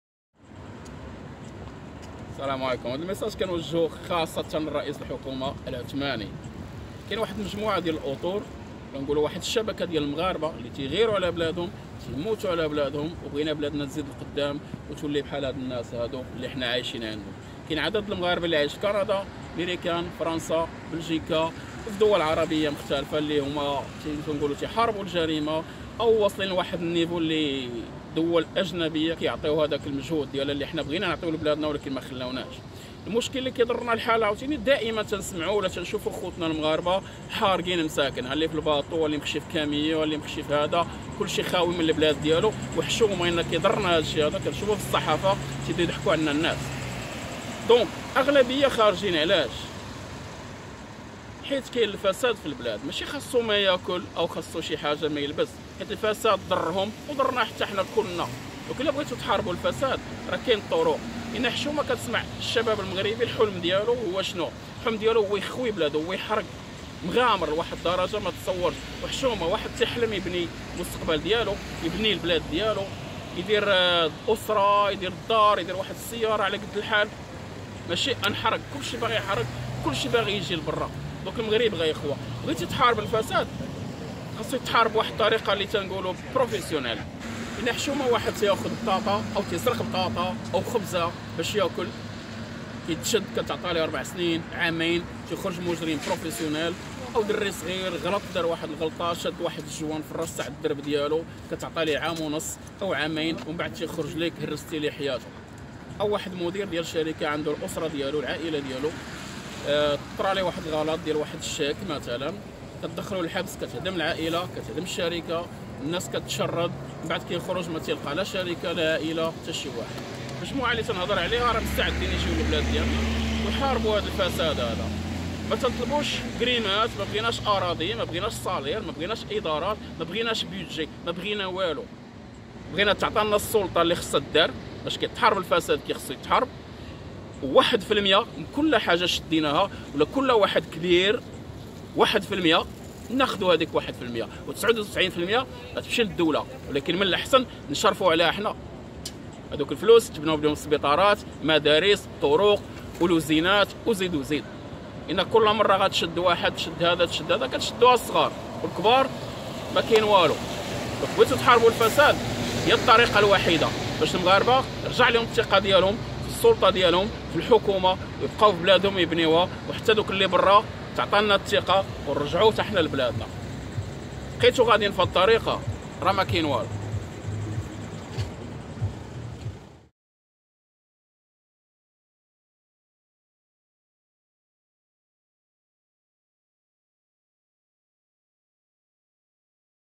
من السويد..المناصفي يرشد رئيس الحكومة العثماني جواز محاربة الفساد
إذا كانت مجتمعاتنا العربية في منأى عن طغيان الفسادفحتما سيكون العالم العربي الأكثر ازدهار
أعود هنا للمثل المعروف :(درهم في الحلال خير من عشرة في الحرام)
يبقى السؤال المطروح :إلى متى سنعيش في هكذا فساد يُعكِّر جو بُلداننا؟؟

Si l'injustice et la corruption n’étaient pas présentes dans les sociétés arabes, le monde arabe serrait le plus puissant sur terre. Pour cela je dis toujours ; mieux vaut un petit sou dans la clarté, que dix dans la saleté. Mais jusqu’à quand allons-nous vivre avec cette folie de corruption qui règne notre monde ?